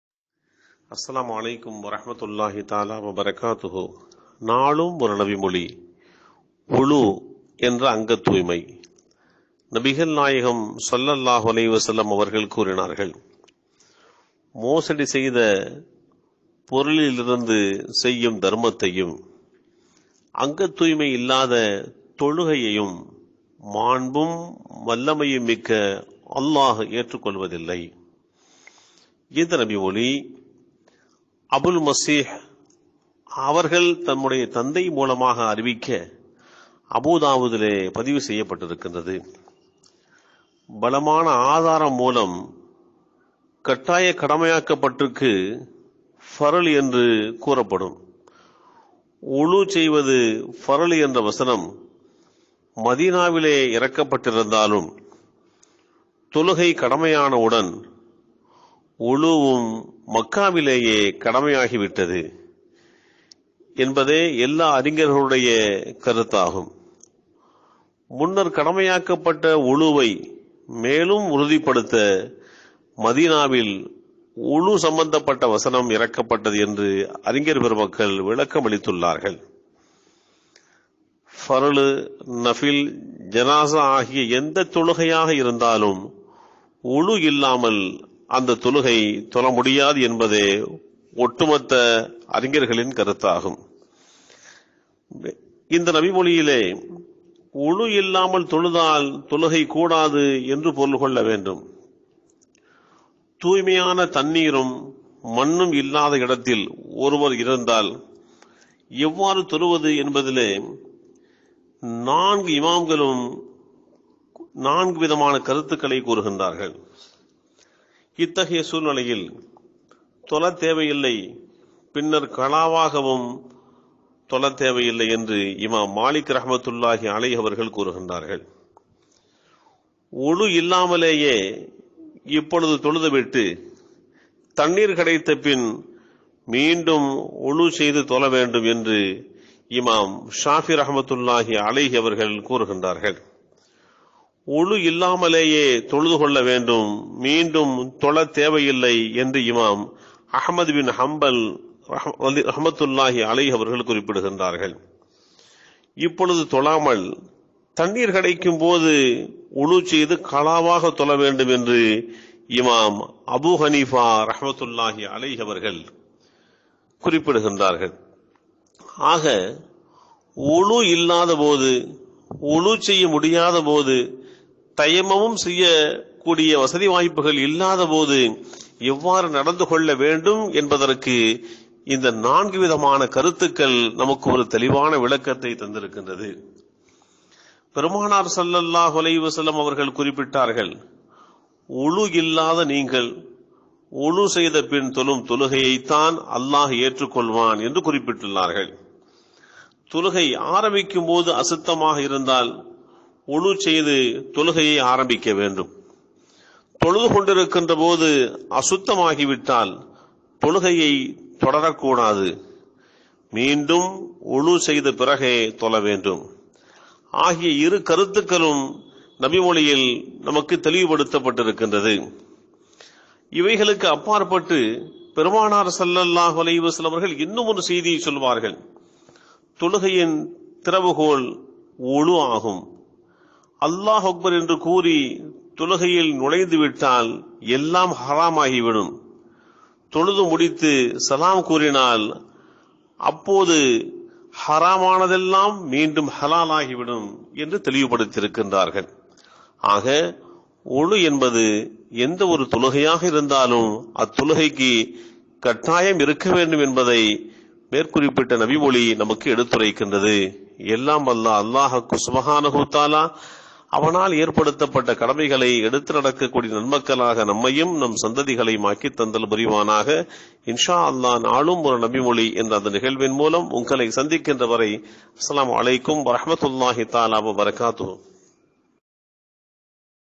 205 - ஒளு [அங்கத்தூய்மை]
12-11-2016
நாளும் ஒரு நபி மொழி
தலைப்பு : ஒளு [அங்கத்தூய்மை]
பயான் : மௌலவி M.K.I. முஹம்மது மன்சூர் அலி
நூரி (கீழக்கரை).